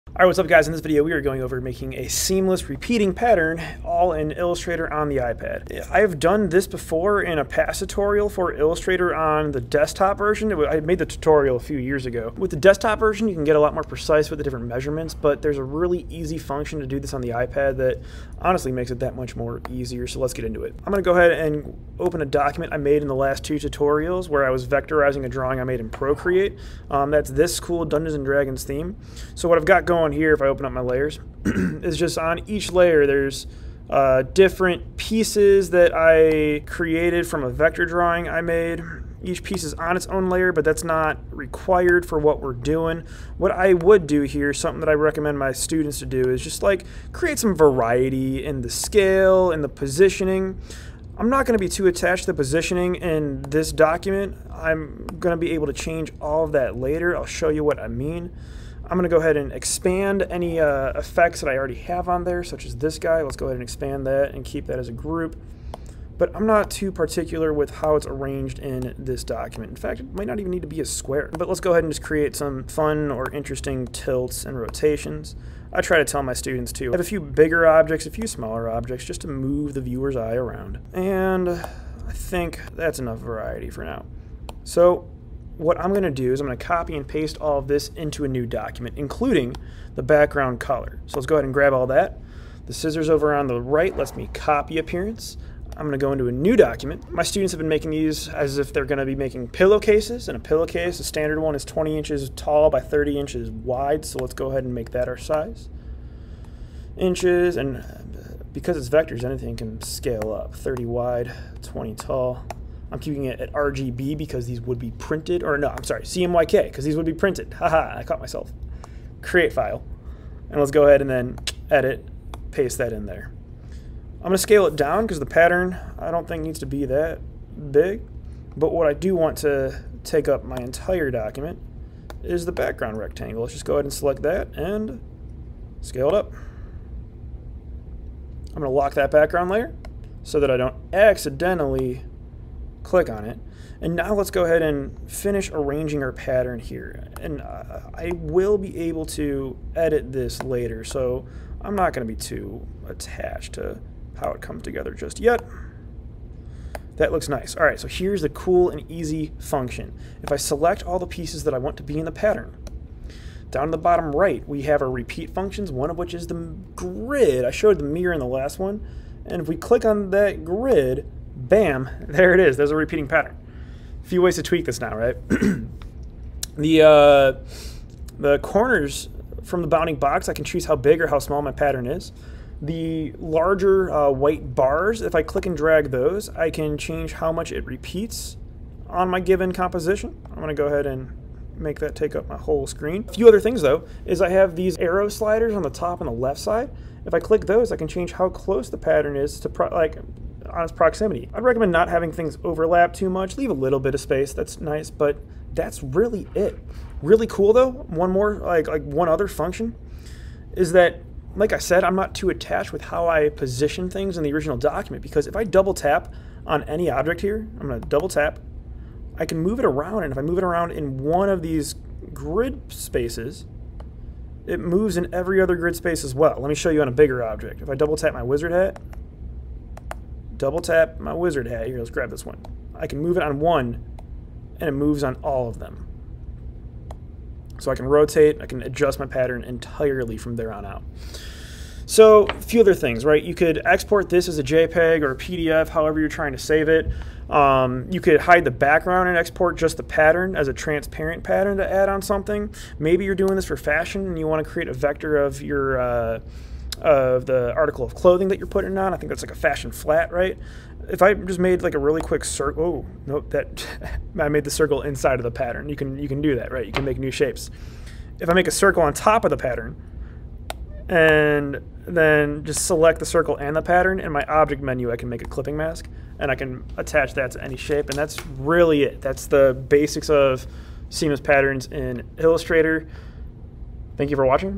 Seamless Repeating Patterns in Illustrator on the iPad
For my Graphic Design students - creating seamless, repeating patterns using Adobe Illustrator iOS!